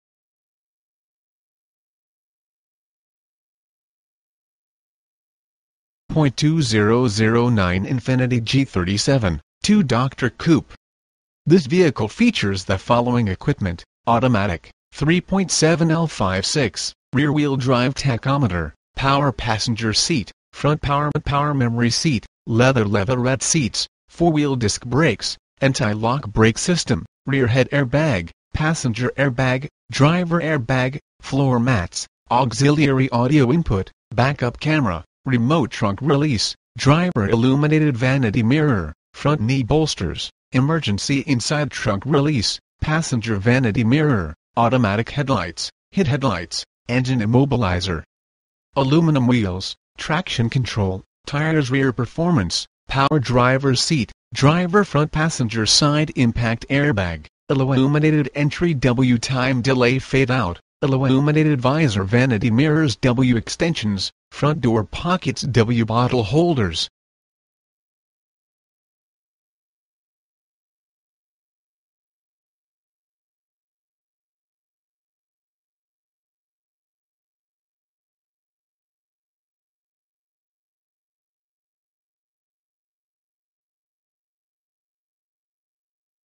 2009 Infiniti G37 - Nissan of the Eastside - Bellevue, WA 98005
For Sale: 2009 Infiniti G37
VIN: JNKCV66E49M723777
Engine: 3.7L V6
Drivetrain: Rear Wheel Drive
Transmission: Automatic
Mileage: 25,271
Color: Liquid Platinum Metallic (exterior) Black (interior)

Features:
2009 Infiniti G37 2 Dr Coupe.